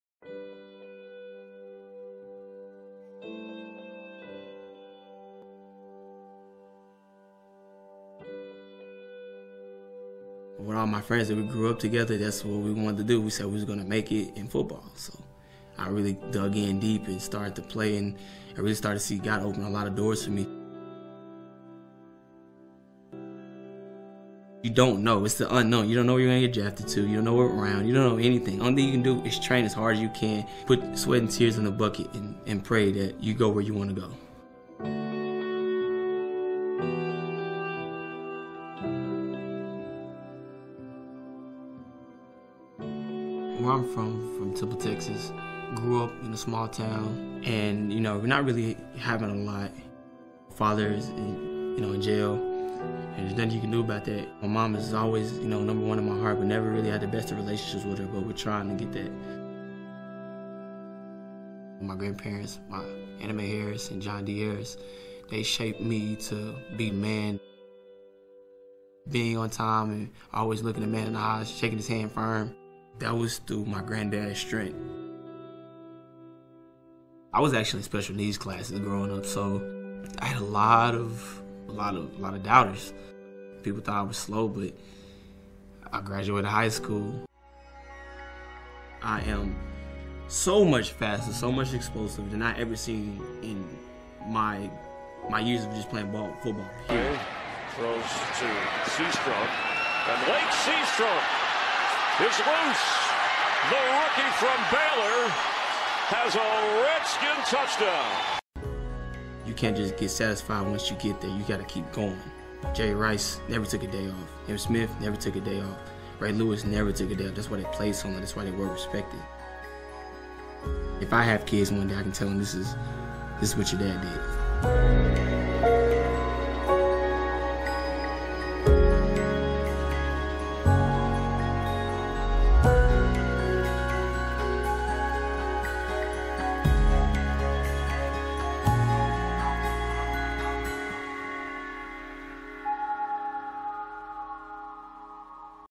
lache seastrunk PYC
Lache Seatrunk NFL Runnning Back talks about the character behind his success